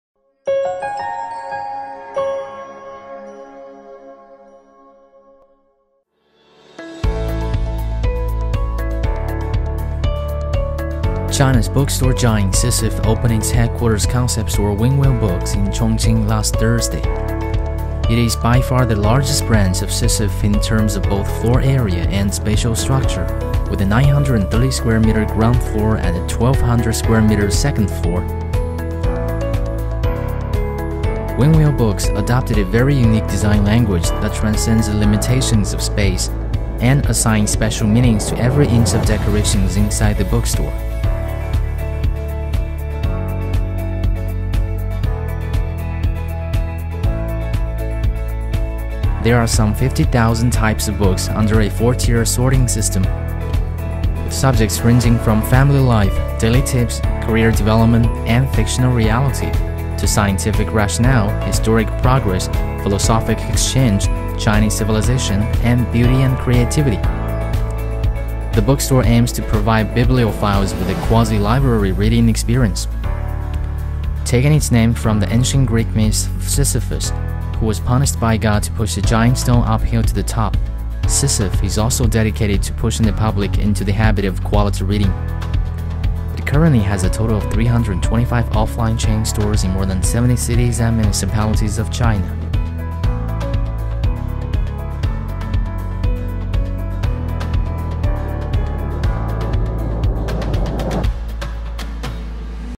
Bookstore Giant Sisyphe Opens Headquarters Concept Store in Chongqing
Sisyphe, one of China’s biggest brick-and-mortar bookstore chains, opened its headquarters concept store Winwill Books on Ruitian Road No. 58 in Yuzhong peninsula’s International Business District bordering the Jialing River last Thursday.
Winwill Books doubles as the new global headquarters office of the Guizhou-born private bookstore chain giant, which currently has a total of 325 offline chain stores in more than 70 cities and municipalities of China. It’s also by far the largest branch of Sisiphe Bookstore Chain in terms of both floor area and spatial structure, with a 930-square-meter ground floor and a 1,200-square-meter second floor.